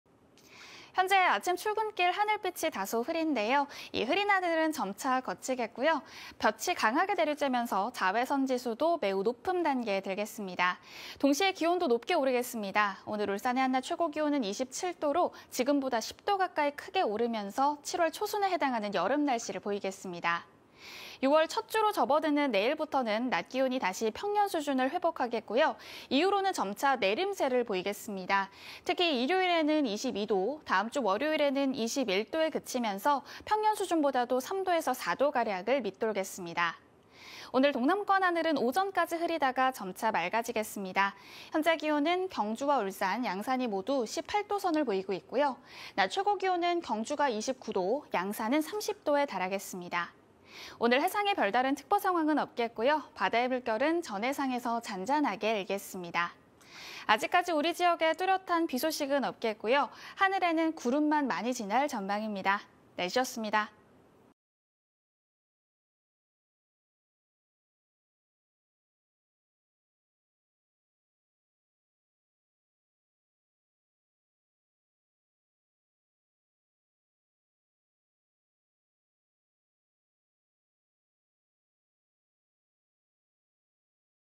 하늘 점차 개고 낮 기온 '쑥↑'.. 자외선 강해 (2024.05.31/뉴스투데이/울산MBC)
현재 아침 출근길 하늘빛이 다소 흐린데요. 점차 흐린 하늘은 걷히겠고요.
볕이 강하게 내리쬐면서, 자외선 지수는 '매우 높음' 단계에 들겠습니다.
기온도 크게 오르겠는데요. 오늘 울산의 낮 최고 기온 27도로,
지금보다 10도 가까이 오르며 7월 초순만큼 덥겠습니다.